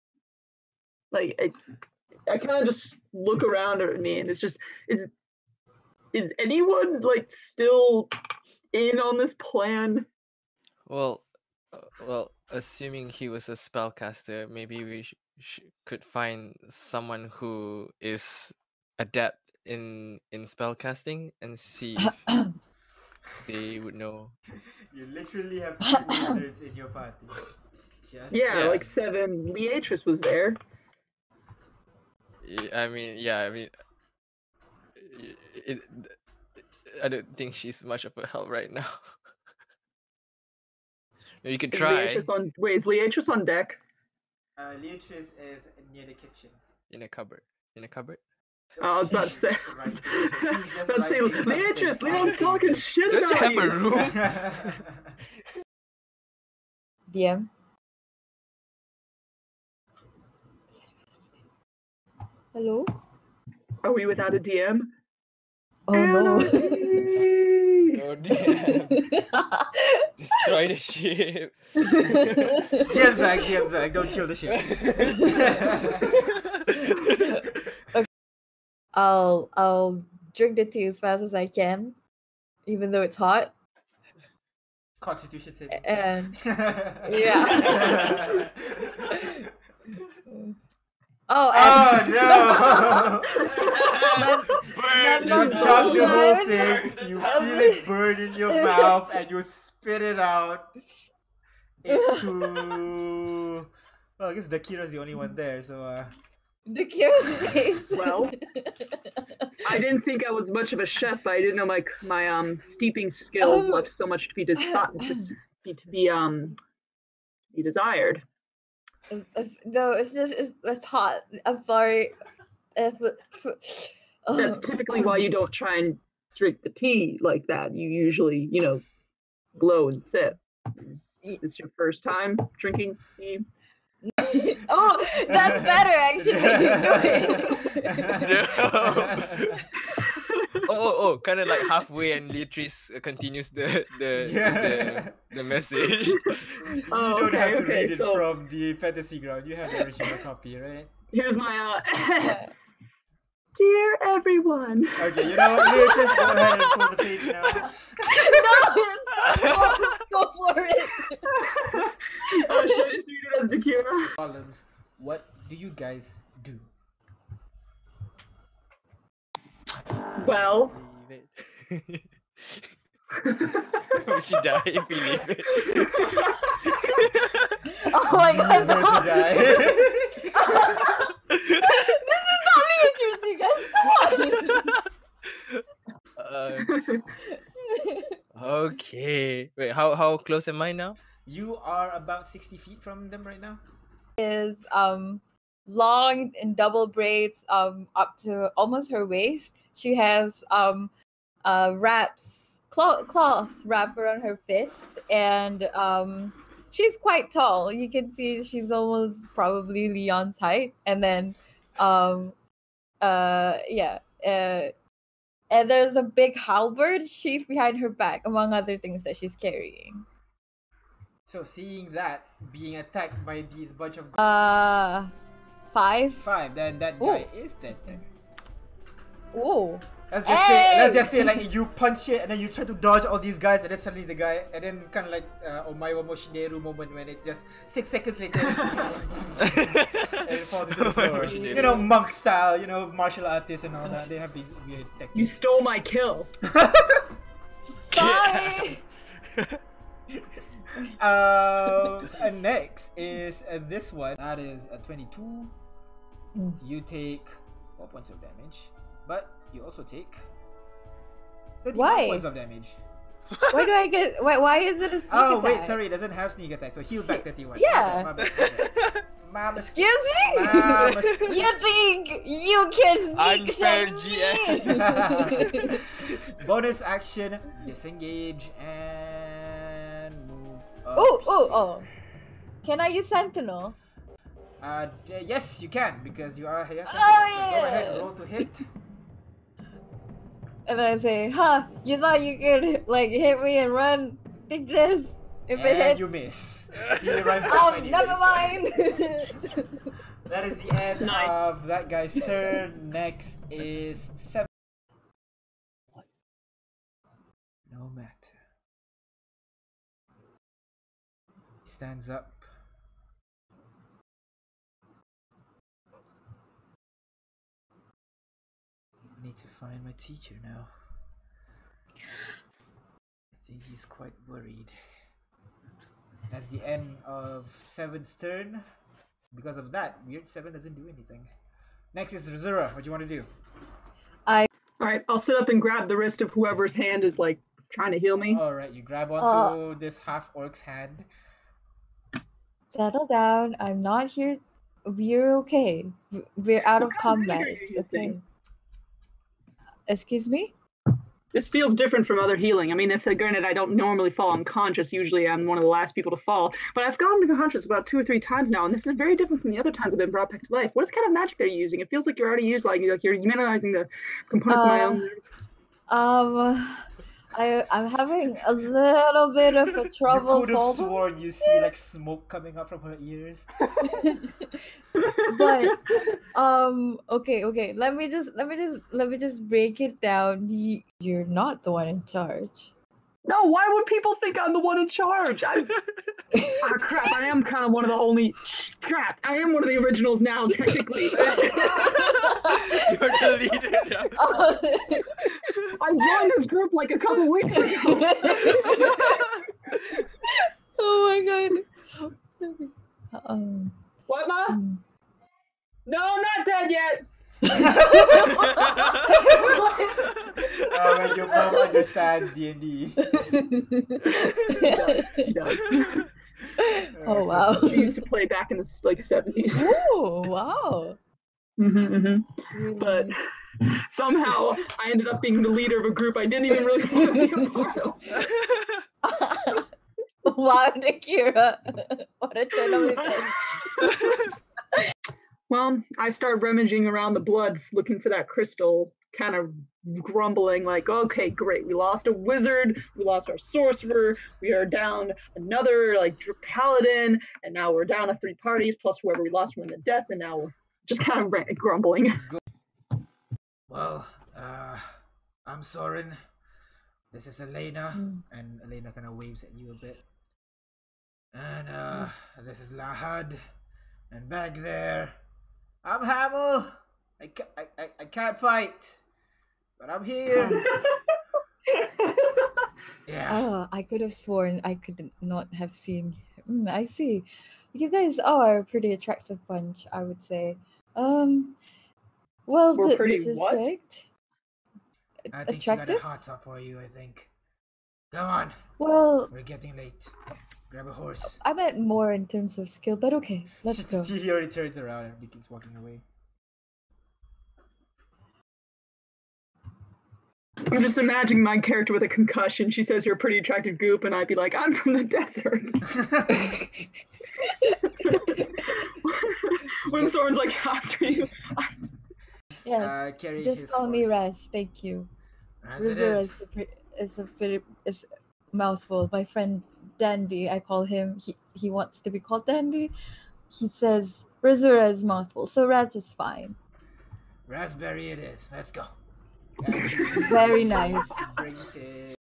Skyllian Rogues Episode 15 Twitch Clips Compilation
Episode 15 was... a lot. The party had to deal with a bunch of changes and the consequences of party members dying or disappearing. But it is not one without interesting and funny clips to share!

Timecodes and links:

1. Party with two wizards wonder who is adept at spellcasting | 0:00

2. D&D with no DM? Anarchy!!! | 0:55

3. Why you shouldn't chug hot tea | 1:18

4. Dhakira reads Liatris' letter in her voice | 2:13

5. Let's leave her to die | 2:44

6. Razura's first appearance | 3:13

7. "Omae wa mou shindeiru" Moment | 3:44

8. You think you can sneak attack meeee... and you miss | 4:14

10. When another person heals the cleric... | 5:34

11. Dhakira the new leader | 6:21

12. Dhakira is not having a good day | 7:15

13. You're all a pretty attractive bunch | 7:33

14. Razzberry it is | 8:32